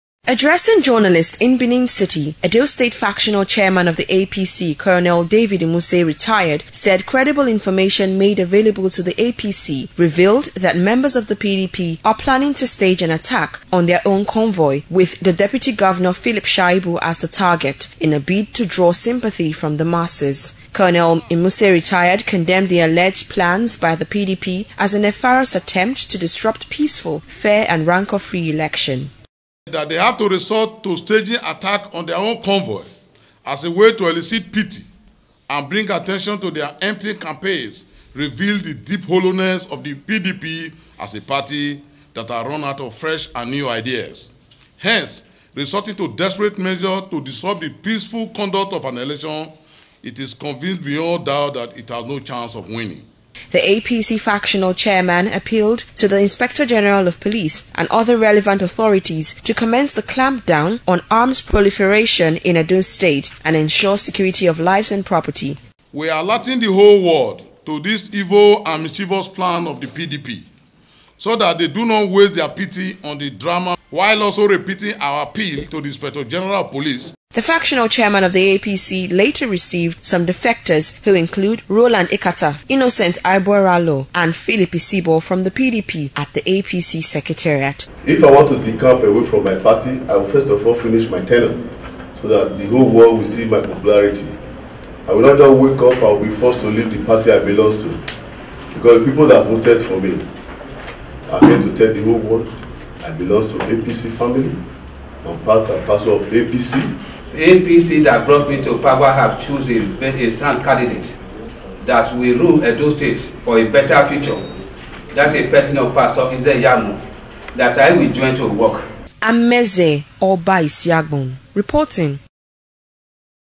Edo APC alleges plans by PDP to frame APC by staging an attack on PDP convoy
The All Progressives Congress, Edo State has accused members of the People's Democratic Party of plotting to frame the APC by staging an attack on the Deputy Governor Philip Shaibu and absolved the APC in connection with the attack.

Factional Chairman of the APC, Colonel David Imuse retired, made the allegation at a press briefing in Benin City.

Amenze Obarisiagbon has details.